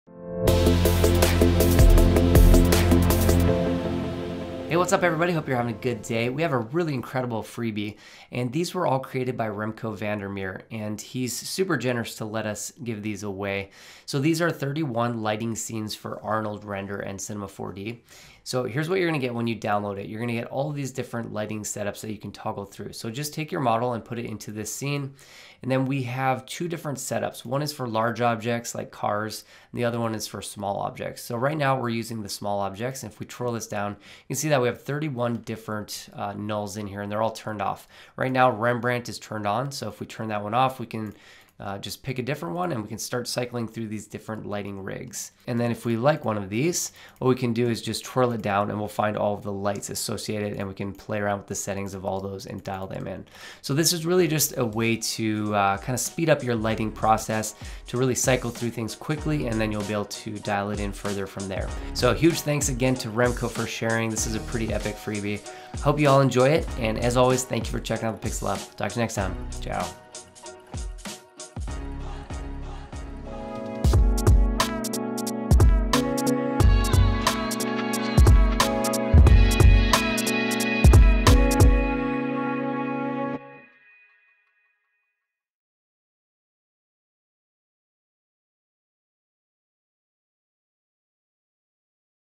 31 FREE Arnold Lighting Studios for Cinema 4D!
I have a phenomenal freebie for you today! It’s a C4D file for Arnold Render which has 31 premade lighting studios! These were created by Remco van der Meer, and he is super generous in giving them away. I’m so thrilled to make these available. You can just add your models to these studios and play with the 31 preset lighting rigs by cycling through the different lighting setups. There’s a lot of variety to experiment with. Enjoy, and a huge thanks to Remco for being so awesome!